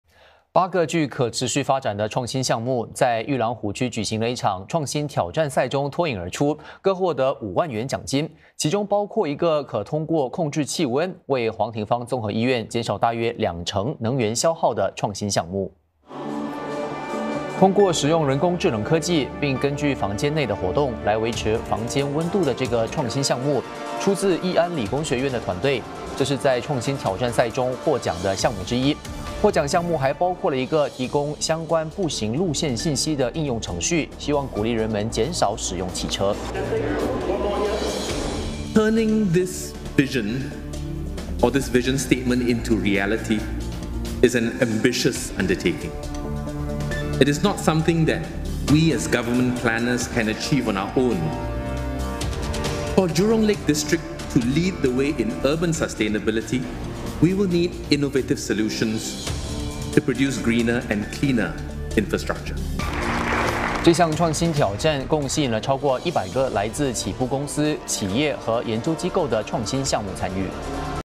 推动裕廊湖区可持续发展 八个创新项目获奖
八个具可持续发展的创新项目，在裕廊湖区举行的一场创新挑战赛中脱颖而出，各获得5万元奖金，其中包括一个可通过控制气温，为黄廷方综合医院减少大约两成能源消耗的创新项目。